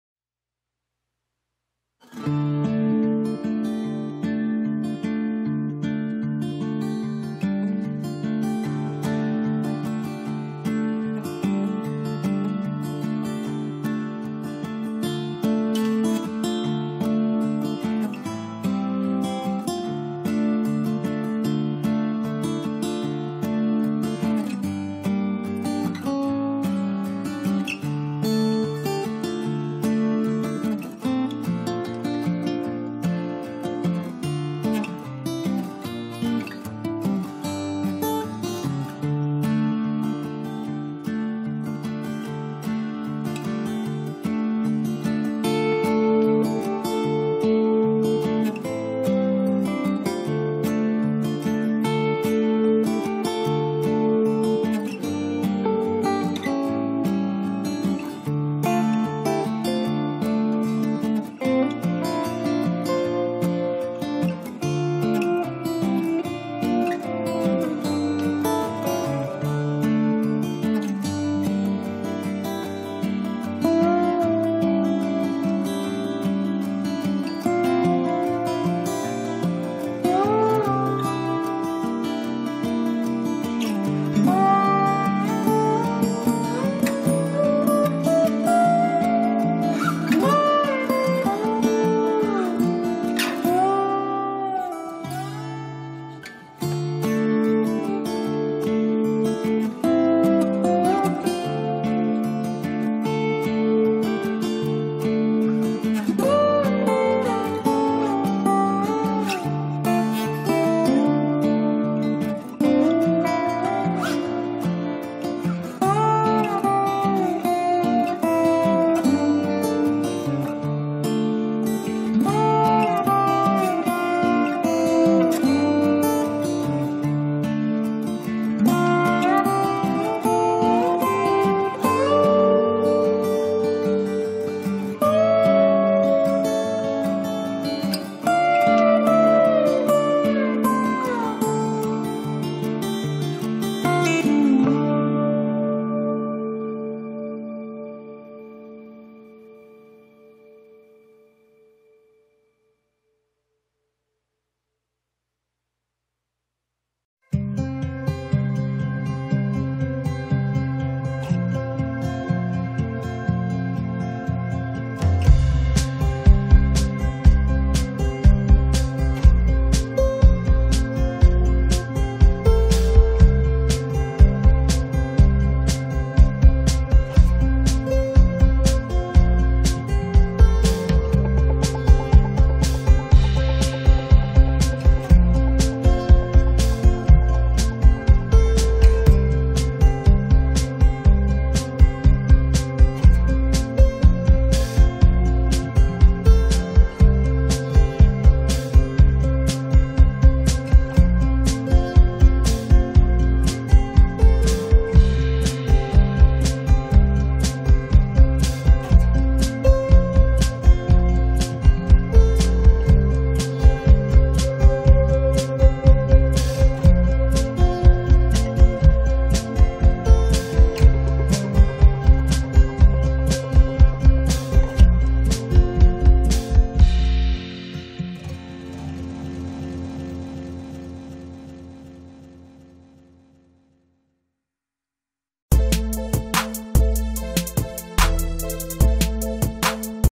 The Top 3 Best Memory Foam Mattress Toppers You Can Buy 2019
3. LUCID 3-inch Gel Memory Foam Mattress Topper - Queen
2. Dreamfoam Bedding 2" Gel Swirl Memory Foam Topper, Queen, Blue
1. Zinus 1.5 Inch Green Tea Memory Foam Mattress Topper, Full